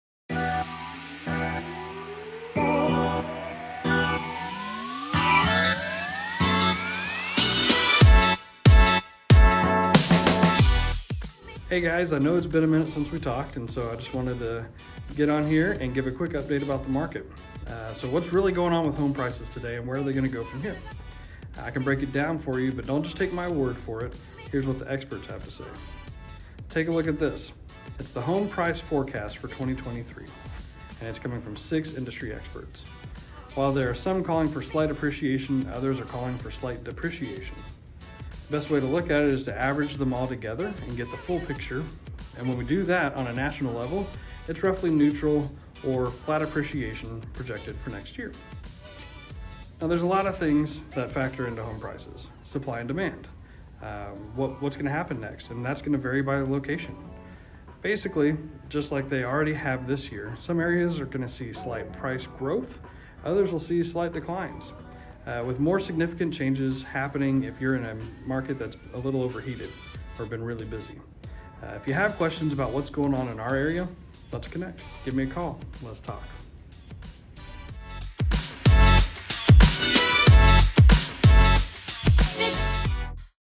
2023 Housing Market
Tuesdays Tips! Let's predict what will happen to home values next year!